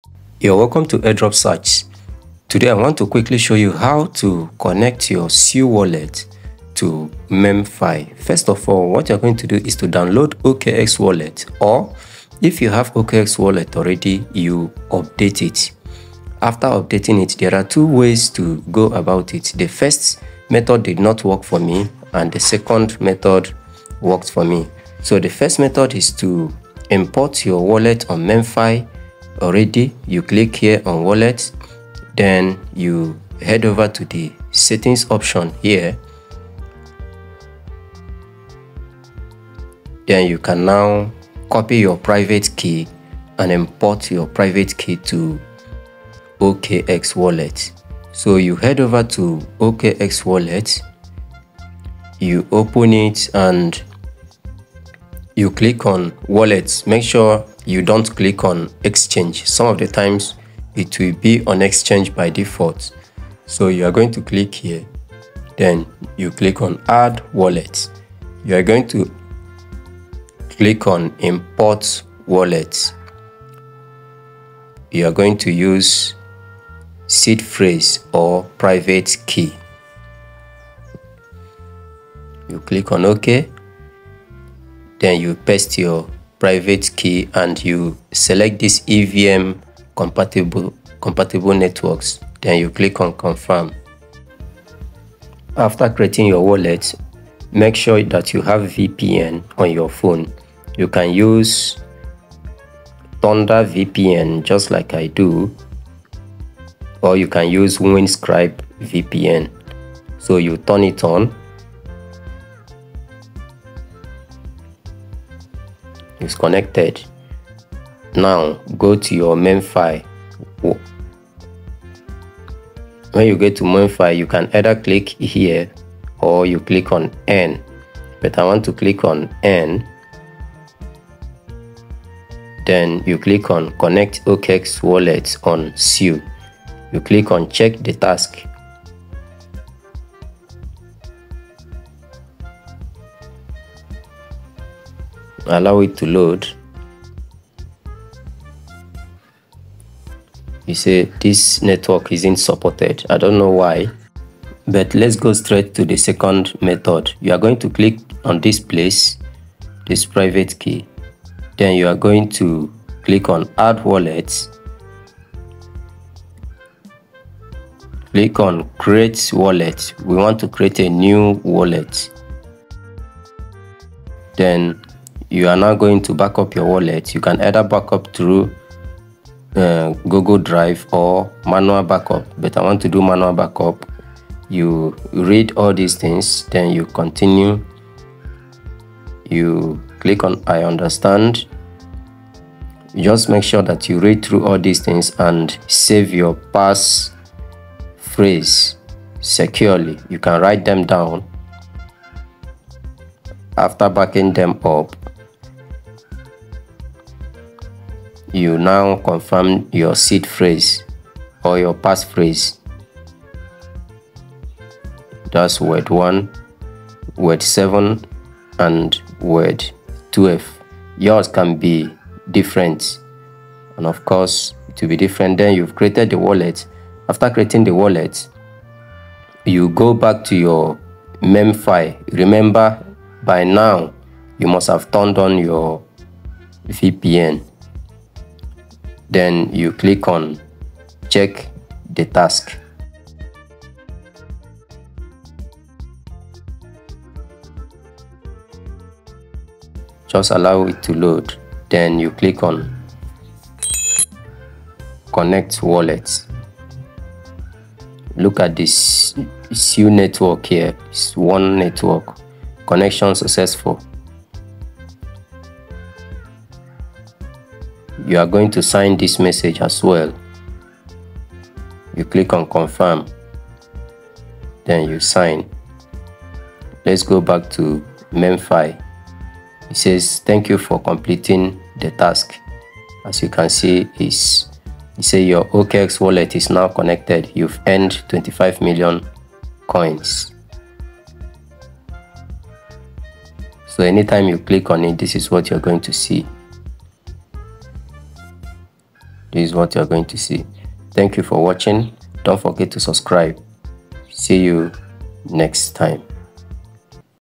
HOW TO CONNECT OKX SUI WALLET TO MEMEFI
Learn how to connect your OKX sui wallet to memefi easily